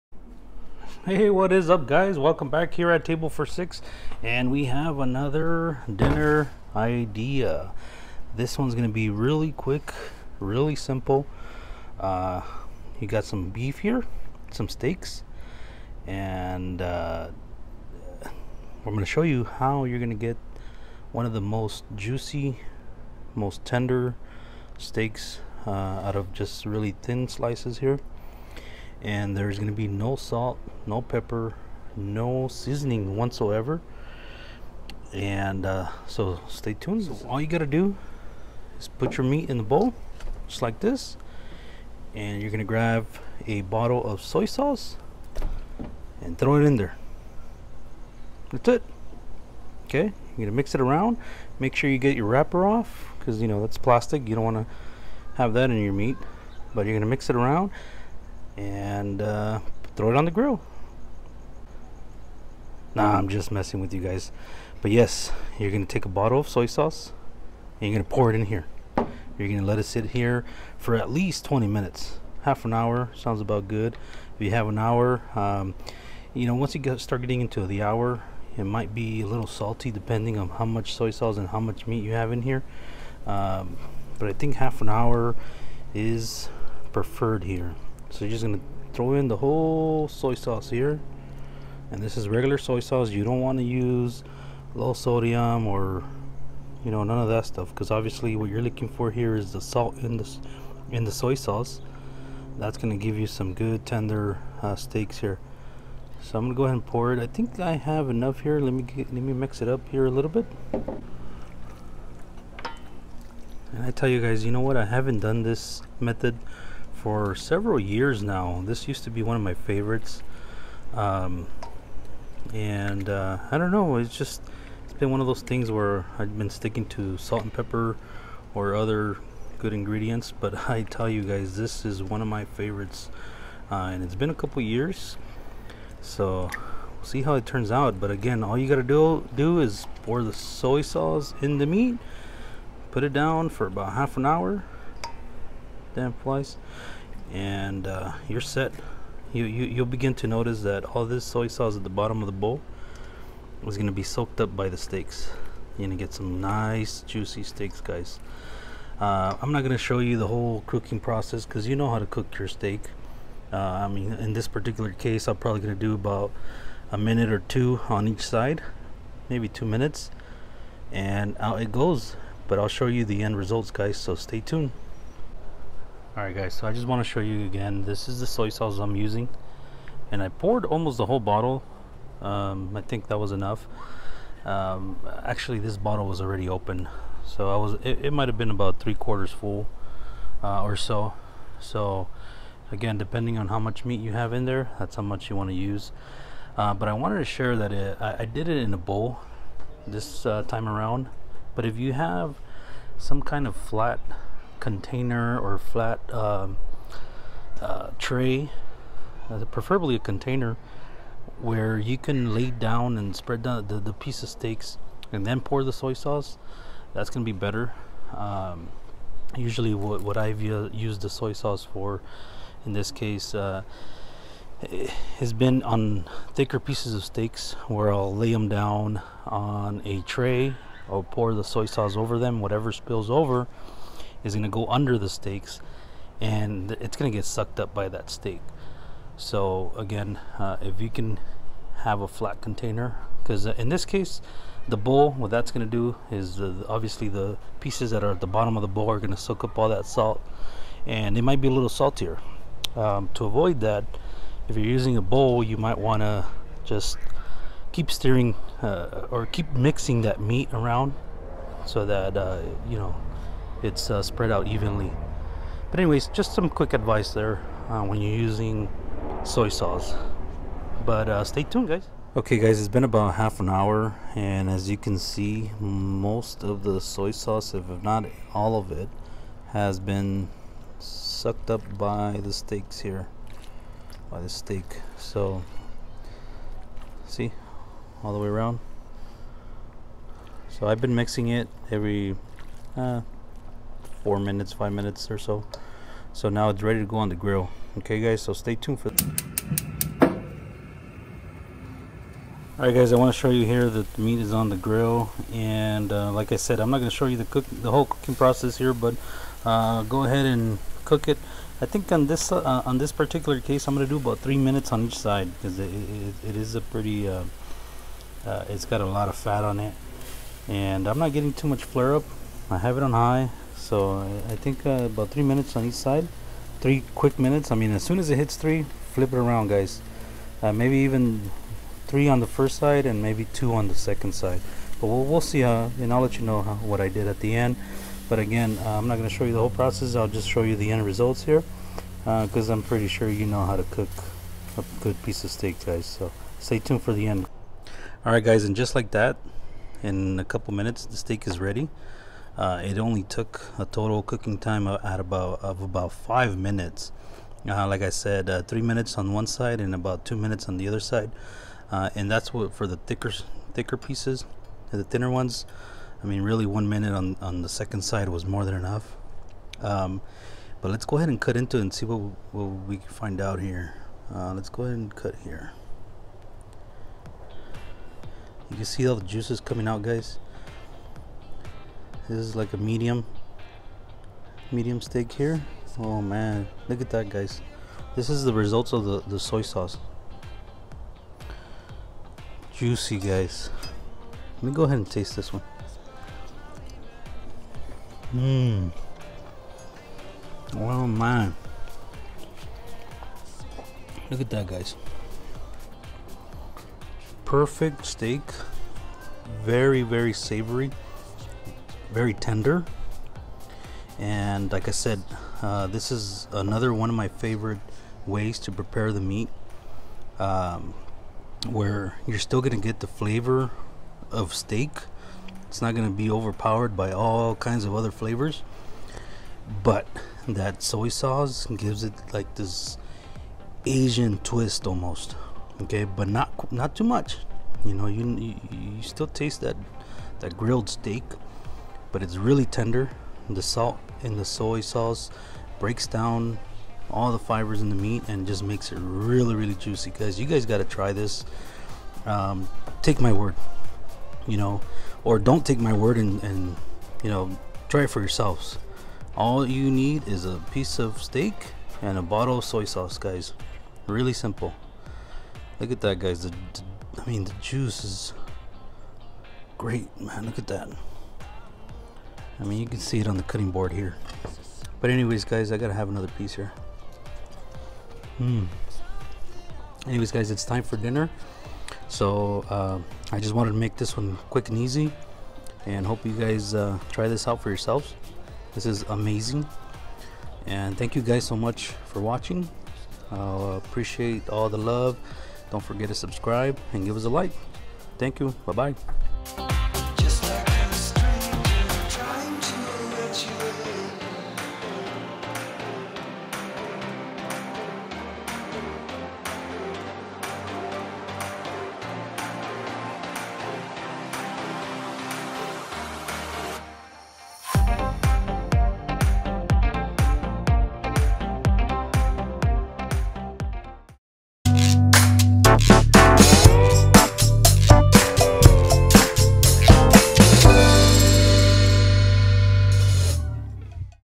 Soy Sauce Steak Marinade - Tender, Juicy Meat
Friends, it's been a long time since I prepare my steak like this. I had forgotten how savory this method is. You must give this a shot. Any size steak will become juicy, tasty and tender. Hope you enjoy this quick and easy dinner idea. Your subscription will keep us going.